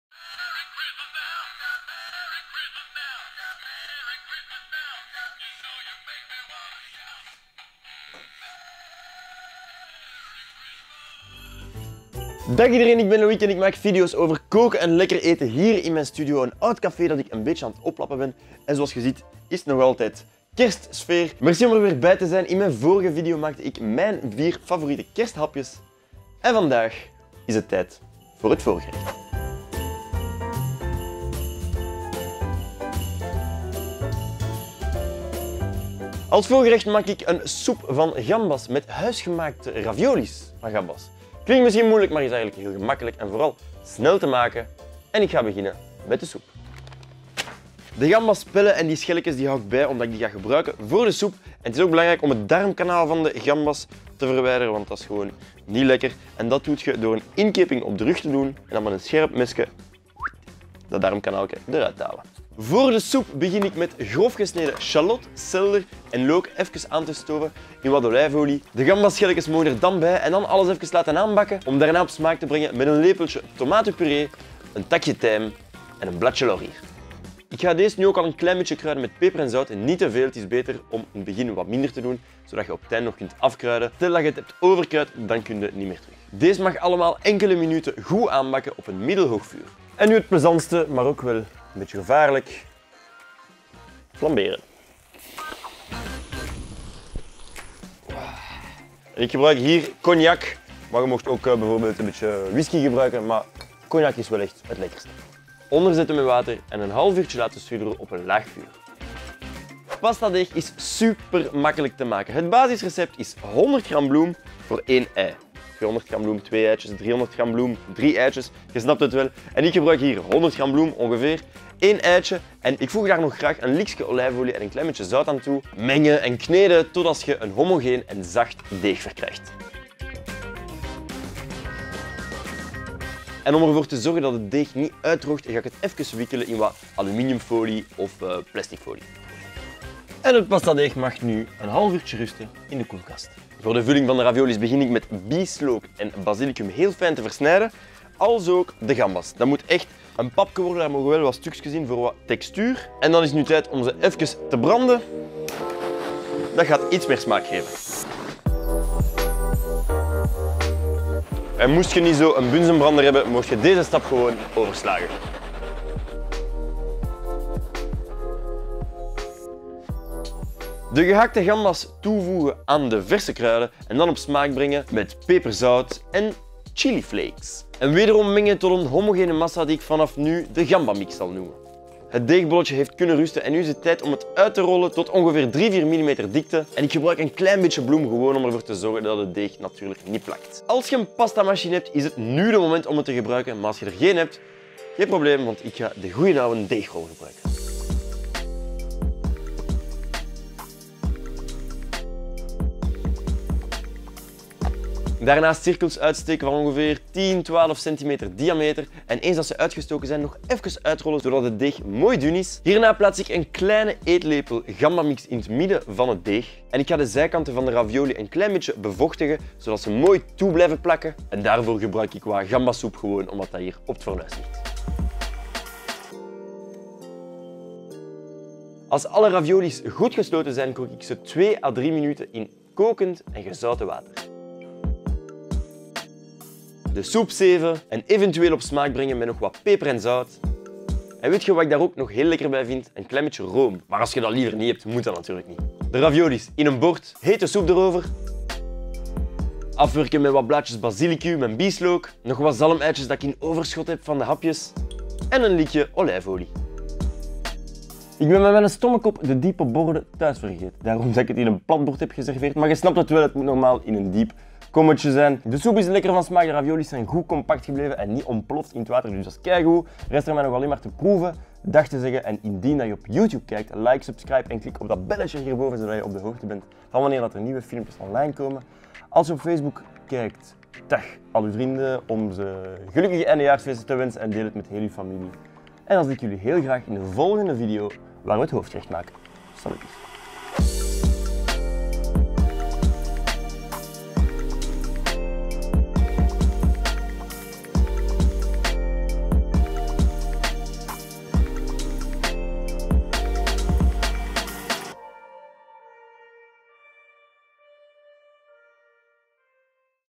Kerstspecial - Het voorgerecht
Als voorgerecht voor mijn Kerstspecial maak ik zelfgemaakte ravioli's gevuld met gamba's in een heerlijke soep :D
🔥🔥🔥 Er komt ook wat vuur bij kijken 🔥🔥🔥

Mensen die het mogelijk maken dat ik onnozel kan doen in mijn keuken en er video's over kan maken.